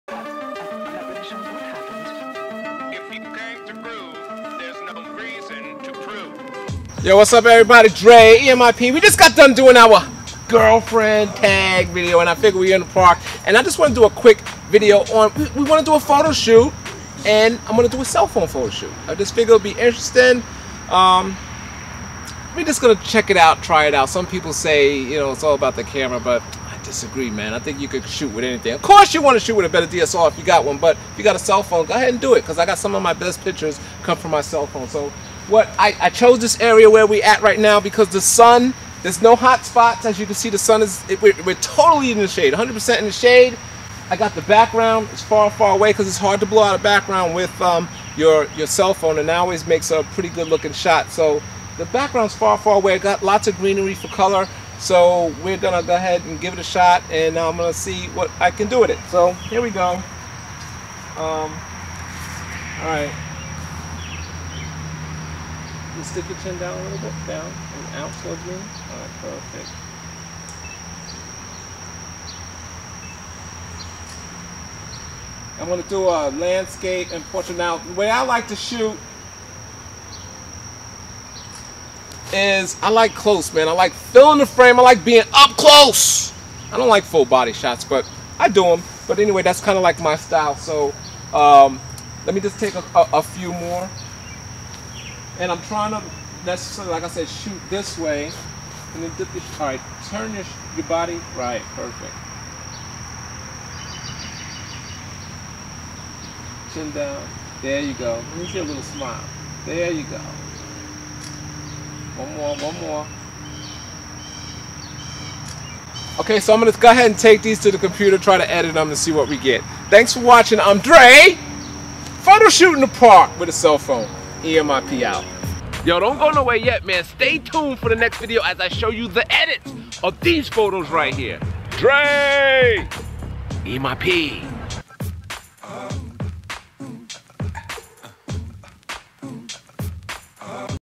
One Minute Photo Shoot
Where I Buy all of my Gear (Best Prices Too!!)
I'm in the park doing an unusual photo shoot with my cell phone.  YES, you can do a photo shoot with cell phone.  You can do a photo shoot with any camera, who says it actually has to be a dslr type camera or one of those mirrorless cameras like the Sony A Series.

I did this QUICK photo shoot for 2 reasons.  One, my camera is making the video for this video so it wasn't available.  Two, I just wanted to show that you can take nice professional type pictures with a cell phone.

Be sure to stay tuned for the next video as I show the final results of this photo shoot and touch a little bit on how I edit them as well.

oh yeah, Don't forget to